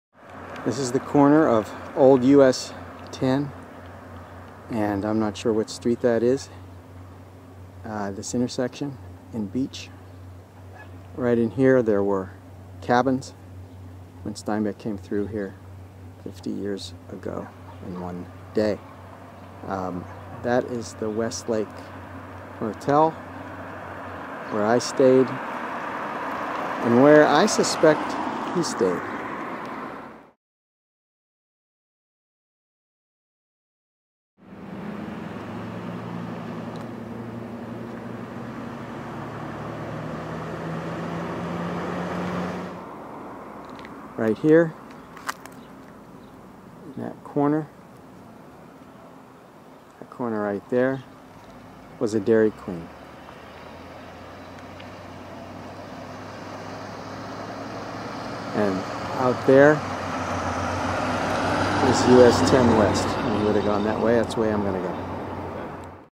10 -- Retracing John Steinbeck's 'Travels with Charley' trip: Sleeping well in Beach, N.D.
Steinbeck had himself a hot bath in the Westgate Motel.

Based on my best drive-by journalism and a follow-up, I've concluded that after a long day of driving across North Dakota John Steinbeck stayed at the Westgate Motel in Beach, N.D., on Oct. 12, 1960. This is a video of the intersection on U.S. 10 that once included a Dairy Queen diagonally across from the Westgate. Steinbeck joked to his wife in a letter from Beach that he was staying at a Dairy Queen motel. (I mistakenly call the Westgate the "Westlake" in this video's narration, but that's because at this point in my trip I was semi-deranged.)

Where Steinbeck Took His Bath
Steinbeck arrived in Beach, North Dakota, on the evening of Oct. 12, 1960. It was the end of Day 3 of his Chicago-Seattle leg. He never mentions his stay in Beach in Charley, obviously, because that was the night he was pretending to be sleeping under the stars near Alice. But he betrayed where he was in a letter to his wife Elaine. Telling her he had stopped at a place that had the only public phone for 40 miles, he joked that he was staying "in a motel called the Dairy Queen.”
He told Elaine that when he went into a bar, all the talk was about deer hunting, not Game 6 of the Yankees-Pirates World Series, which he had listened to that day on his truck’s AM radio. He could have walked over to the Bijou Theater a few blocks away and seen Alec Guinness in Our Man in Havana. But Steinbeck probably didn't take in a movie for the same reason I didn't go to the Bijou to see Angelina Jolie star in Salt. We had both spent our days driving across North Dakota and were both pretty beat.
As part of my pre-trip research, I had called the only motel I could find online in Beach, the Westgate Motel on Old Highway 10. The manager, Sandy, told me there was never a Dairy Queen Motel in town but there had been a Dairy Queen franchise. It used to be right across the street. By the time I arrived in Beach, 50 years and one night after Steinbeck did, I had come to realize he had been kidding his wife about staying in a motel called the Dairy Queen. But what motel was it? Was it the “modern” Westgate, the obvious choice, where Sandy had kindly saved me one of her 11 rooms without my asking? Or did Steinbeck stay in one of the old-style motel cabins that once stood on the vacant lot across from the Westgate?
I spent the next early morning sleuthing around Beach. Based on a random drive-around, Beach was a mix of good homes and a few shabby ones in mostly older neighborhoods. It still seemed to have everything it needed for a town that had shrunk from 1,460 people in 1960 to 1,019 in 2010. Tragically, no Walmart was closer than 60 miles. But there were several churches, a couple of banks, a county courthouse, a hardware store, a pharmacy, a supermarket, a few shaky restaurants. Beach had a busy railroad line through its heart and one of those cool old water towers that looks like a fat rocket taking off. It even had some history. General Custer and his doomed column came through Beach on their way to immortality at Little Big Horn.